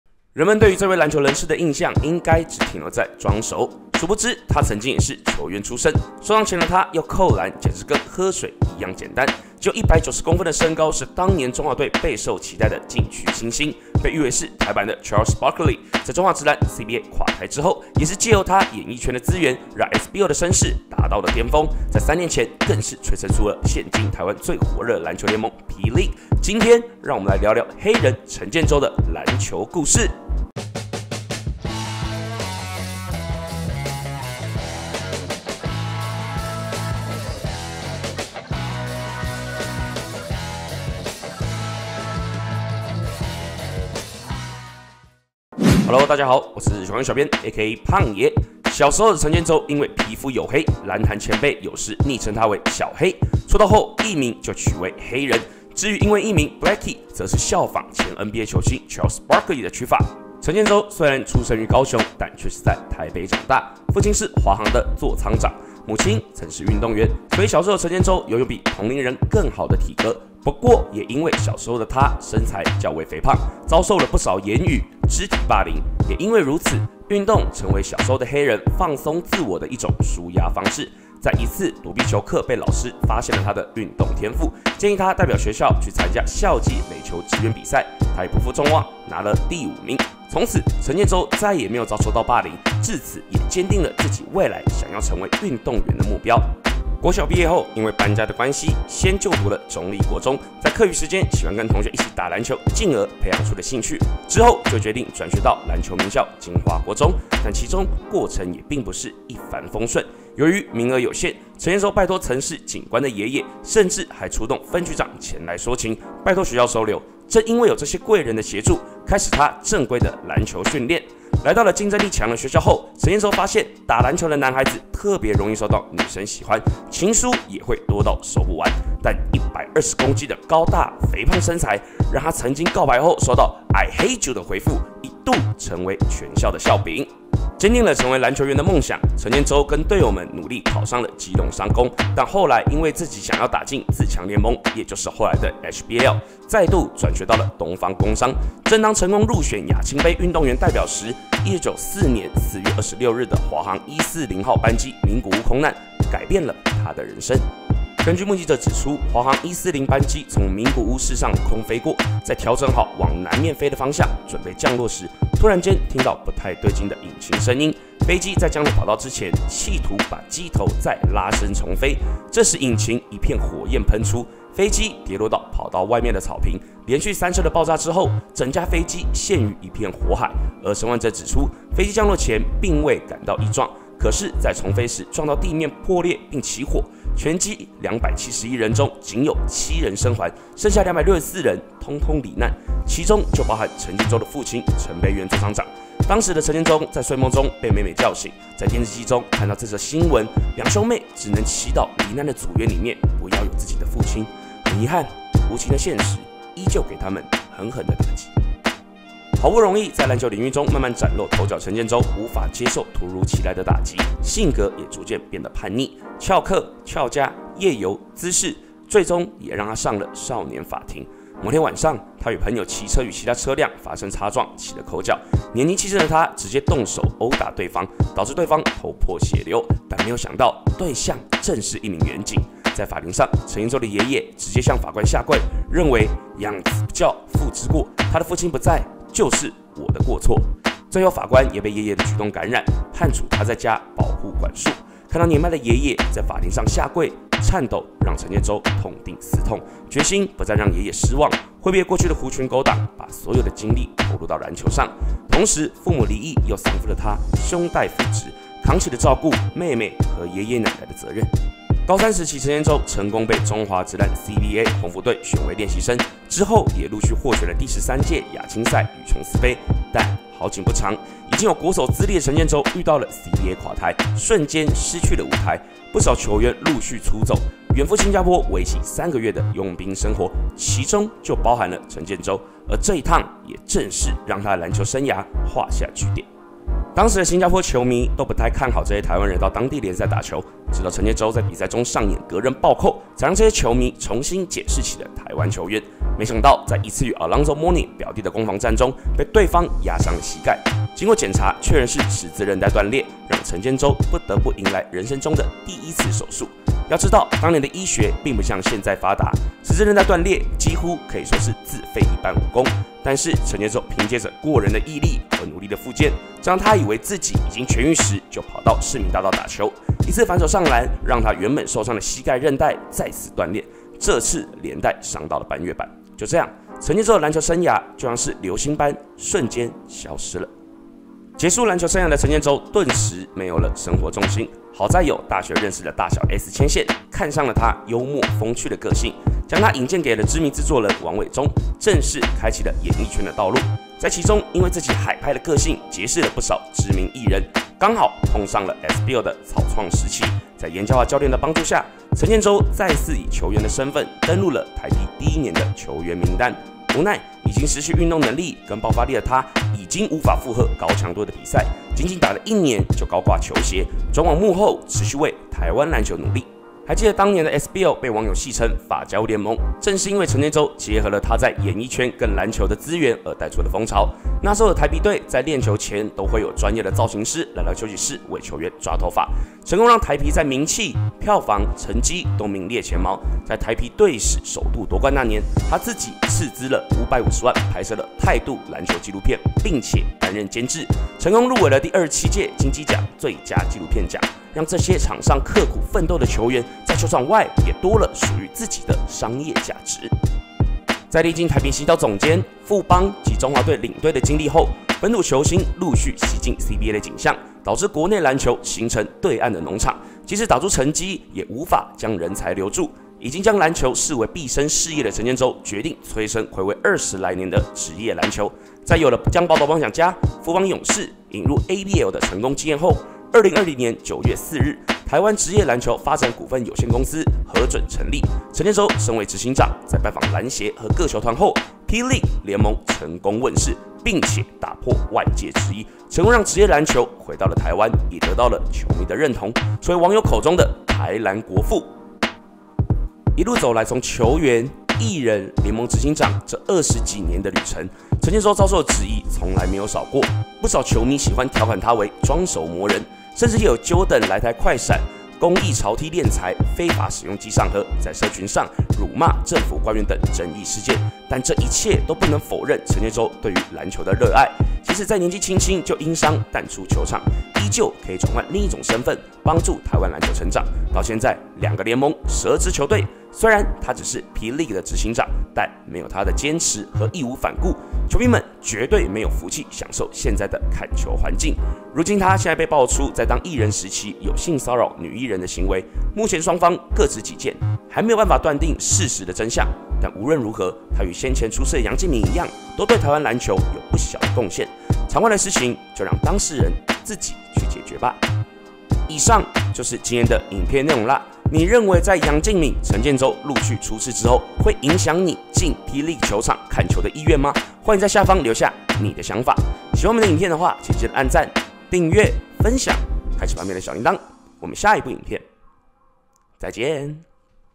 8分鐘帶你認識「台籃國父」陳建州｜爺爺為他向法官下跪 ? ｜曾到新加坡打球求生 ?｜被NBA球星的表弟結束籃球生涯｜拍籃球紀錄片能入園影展 ?｜【球關注人物誌】
人們對這位籃球人士的印象應該只停留在「裝熟」，孰不知他曾經也是球員出身，受傷前的他要扣籃簡直跟喝水一樣簡單，僅有190公分的身高是當年中華隊備受期待的禁區新星，被譽為是台版的Charles Barkley，在中華職籃CBA垮台之後，也是藉由他演藝圈的資源，讓SBL的聲勢達到了顛峰，在三年前更是催生出了現今台灣最火熱的籃球聯盟P League，今天就讓我們來聊聊「黑人」陳建州的籃球故事。

✏️文本：胖爺
✏️縮圖：內湖科比布萊恩
✏️影片：Mouse